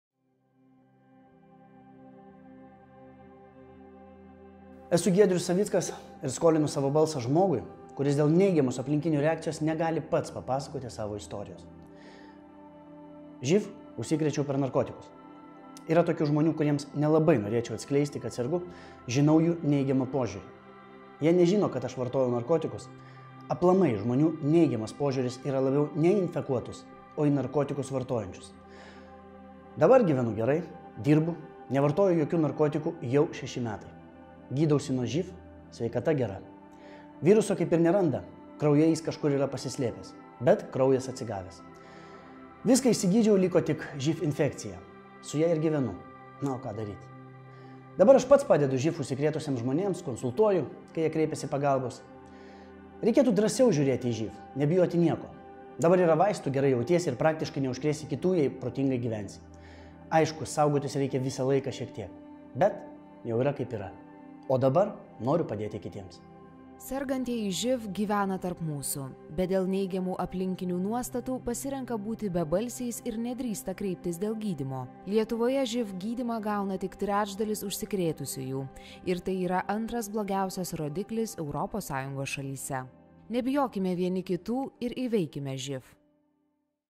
Giedrius Savickas: yra daug žmonių, kurių sąžinė ne švaresnė negu gyvenančiojo su ŽIV
„Žmonės paprastai bijo pasakyti, kad serga ŽIV, bet aš nematau dėl to nieko nešvaraus. Yra daug žmonių, kurių sąžinė ne švaresnė negu gyvenančiojo su ŽIV. O šiaip norėčiau, kad daugiau nebereikėtų ateiti filmuotis, kad socialinė iniciatyva pavyktų ir žmonės suprastų, jog ŽIV nėra nieko baisaus“, – teigia aktorius Giedrius Savickas.

Nors Lietuvoje dažniau ir atviriau kalbama apie ŽIV, patys sergantieji vis dar vengia viešai dalintis savo istorijomis. Apie diagnozę nutylima net artimiausios aplinkos žmonėms, bijantis jų neigiamos reakcijos ir atstūmimo. „Skolinu savo balsą“ — tai šviečiamoji socialinė iniciatyva, kuria siekiama mažinti su ŽIV susijusią stigmą ir sergančiųjų socialinę atskirtį, bei kartu paskatinti juos kreiptis pagalbos.

Lietuvoje gydymą gauna tik trečdalis užsikrėtusiųjų. Ir tai yra antras blogiausias rodiklis Europos Sąjungos šalyse.